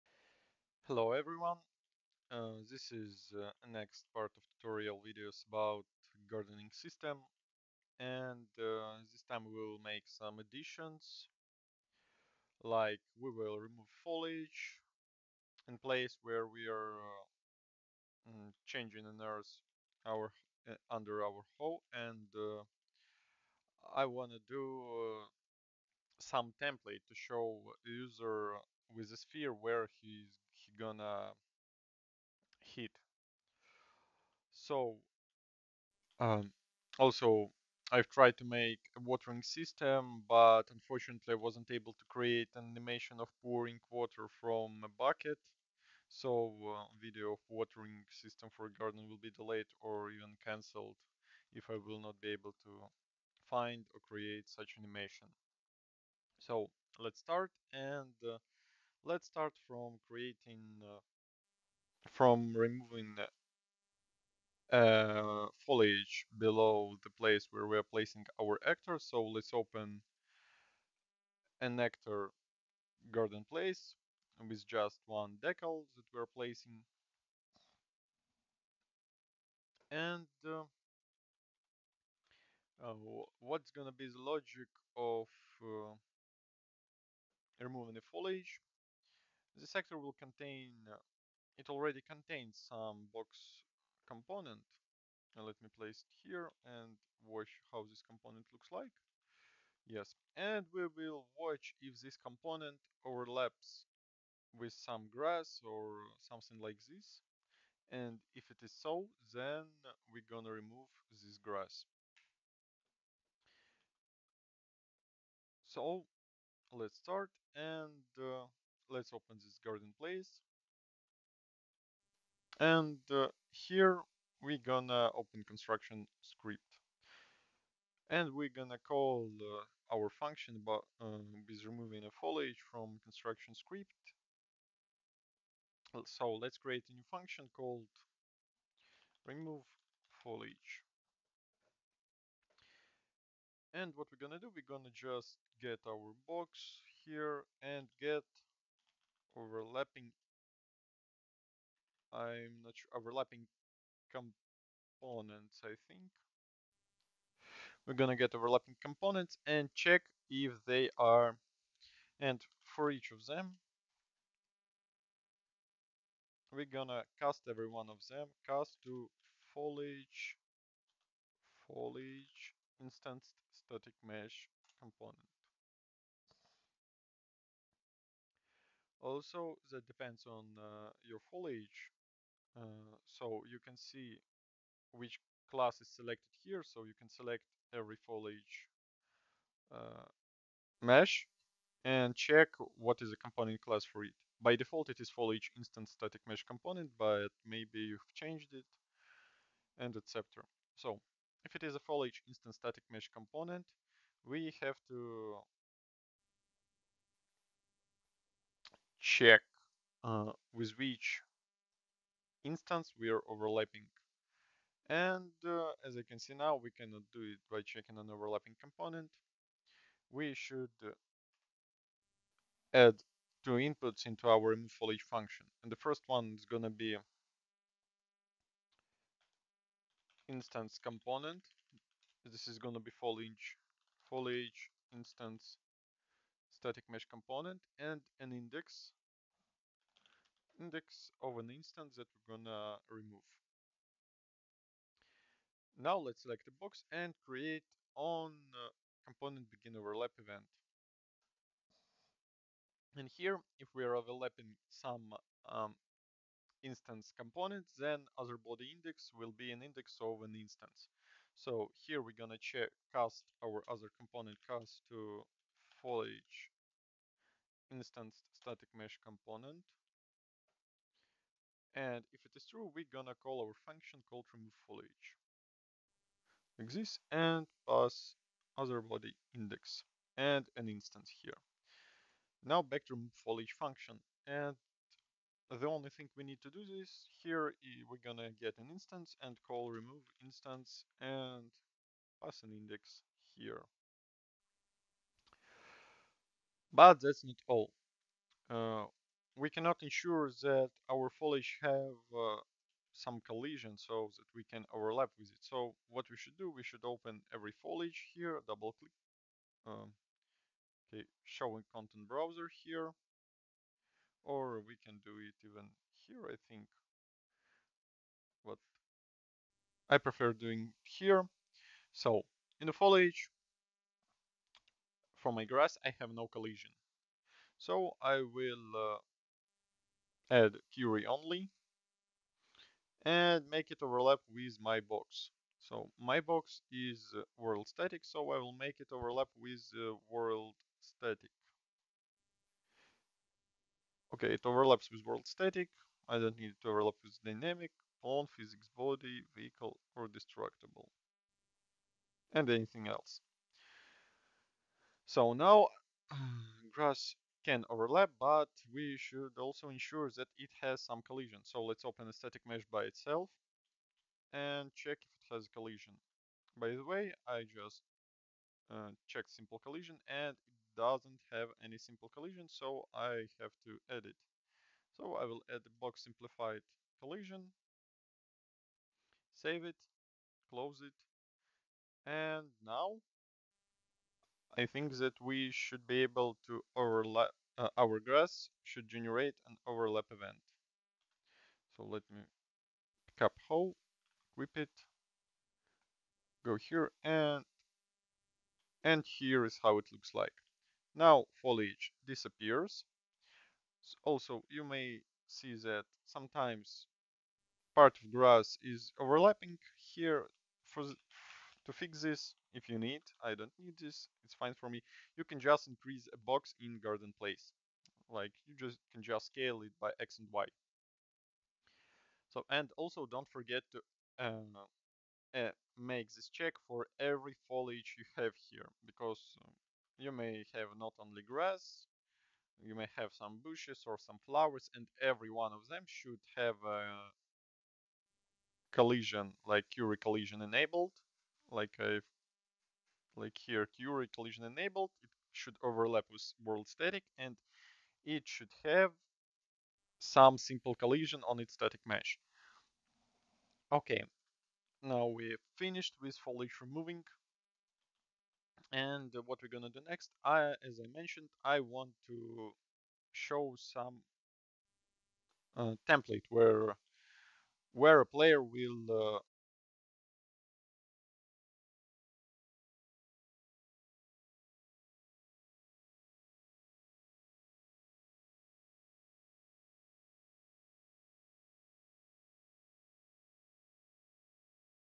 Unreal Engine 5 Tutorial - Growing Plants Final: Remove Foliage below garden
Removing a foliage grass at a place where we placing our garden

This is a reupload of a previous video

!!! NOTE: This is not a begginer tutorial. I expect you to know the basics of Unreal Engine 5 and will not describe every step of creating a system.
!!! NOTE 2: Gardening system is a complex system, that's why we can have some issues and bugs, and it may require some changes from your side to fit your needs.